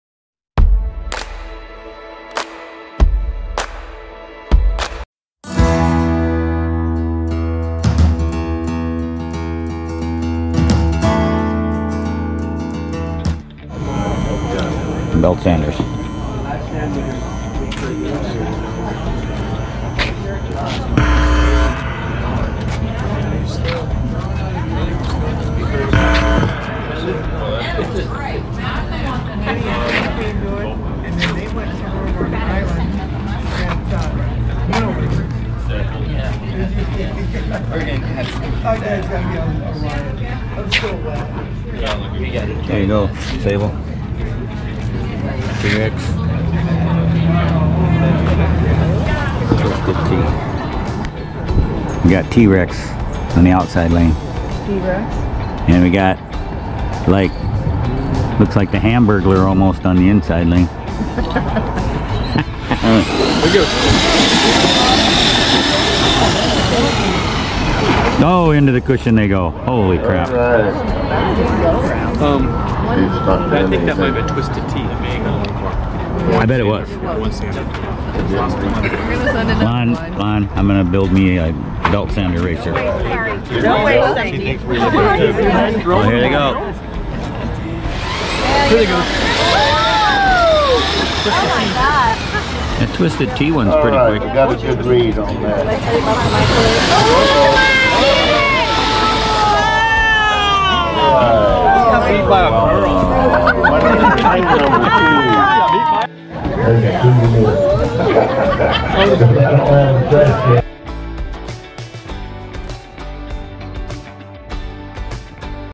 Living Our Best Life, Port Aransas Fulltime RV Fun
Aransas Pass Belt Sander Races, crazy fun time